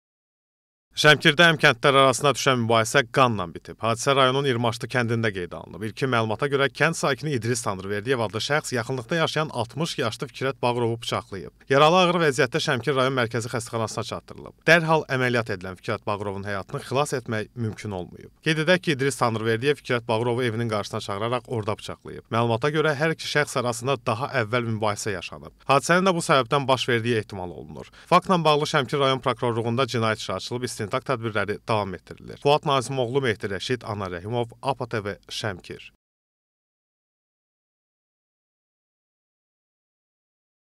Bıçaqlanan şəxs əməliyyat olunsa da... – APA TV
Şəmkirdə həmkəndlilər arasında düşən mübahisə qanla bitib. Hadisə rayonun İrmaşlı kəndində qeydə alınıb. İlkin məlumata görə kənd sakini İdris Tanrıverdiyev adlı şəxs yaxınlıqda yaşayan 60 yaşlı Fikrət Bağırovu bıçaqlayıb. Yaralı ağır vəziyyətdə Şəmkir rayon Mərkəzi Xəstəxanasına çatdırlıb. Dərhal əməliyyat edilən Fikrət Bağırovun həyatını xilas etmək mümkün olmayıb. Qeyd edək ki, İdris Tanrıverdiyev Fikrət Bağırovu evinin qarşısına çağıraraq orda bıçaqlayıb. Məlumta görə hər iki şəxs arasında daha əvvəl mübahisə yaşanıb. Hadisənin də bu səbəbdən baş verdiyi etimal olunur. Faktla bağlı Şəmkir rayon Prokroluğunda cinayət işi açılıb,instintaq tədbirləri davam etdirilir.

Fuad Nazimoğlu
Mehdi Rəşid
APA TV Şəmkir